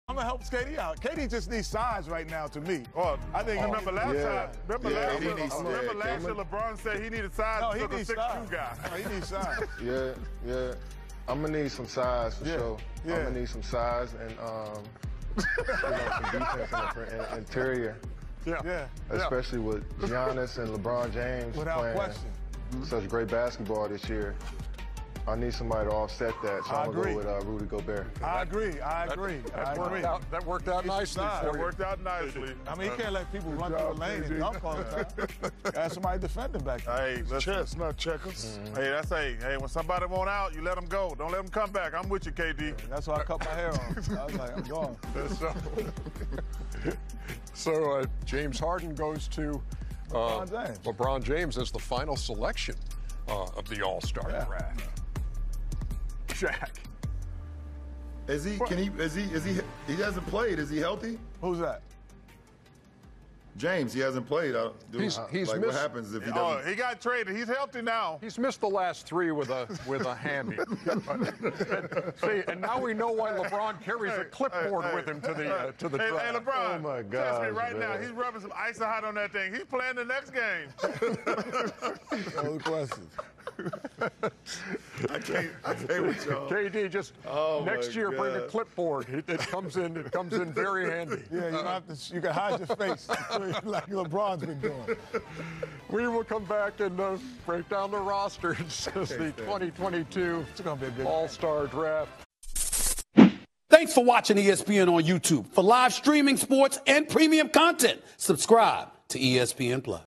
Lebron Laughs at KD not picking James Harden on his All Star Team TOO FUNNY KD keeps a straight face
INDY SPORTS FILM #1 Youth Media Platform in Indiana!

Event coverage and inquires.